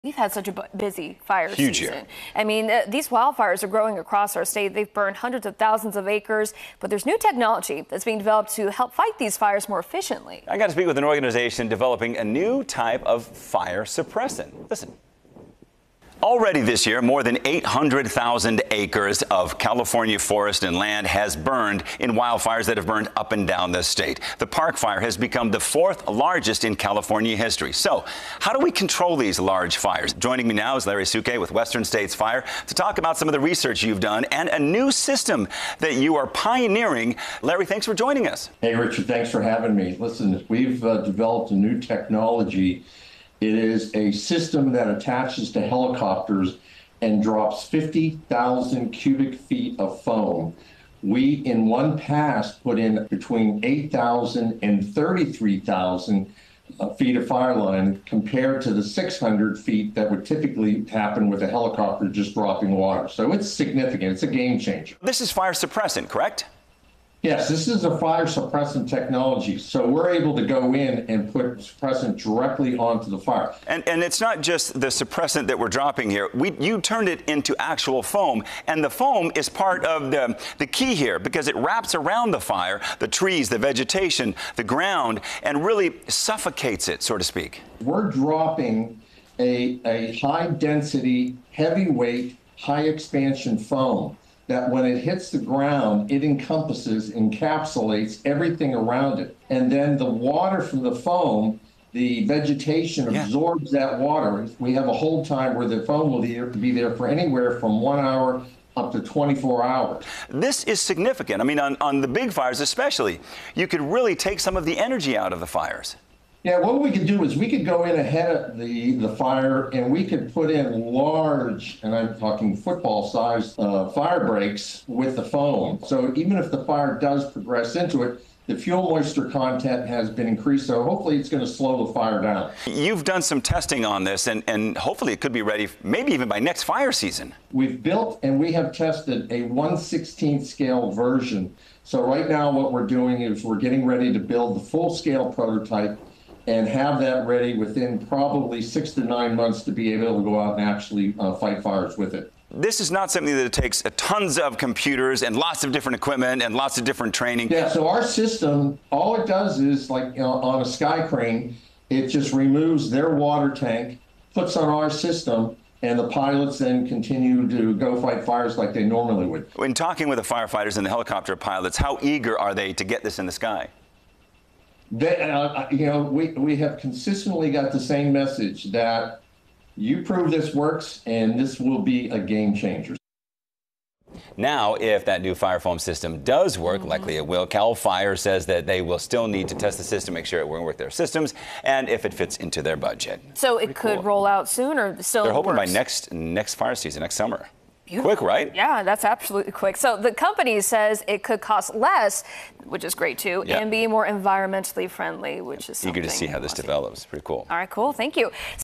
New fire suppression technology being developed to battle California's wildfires
Founder of Western States Fire and developer of SkHI-EX Larry Sukay, discusses how SkHI-EX is the next level in wildfire suppression technology and how it will change the future of firefighting.